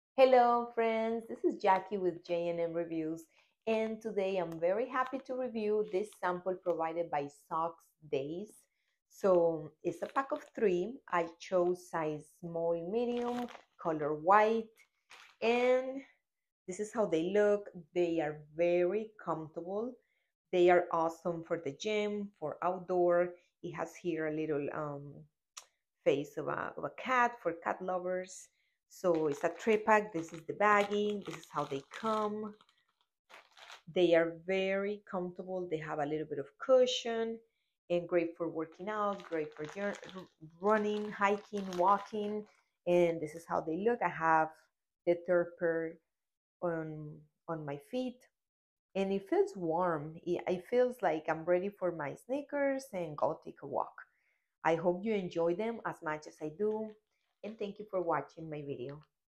Review of Socks Daze 3/6 Pack Men's Women's Merino Wool Ankle Running Athletic Socks
Hire us to make YOUR next video for your brand on Fiverr!

Prime Commission Club

All the tools we use to make our videos!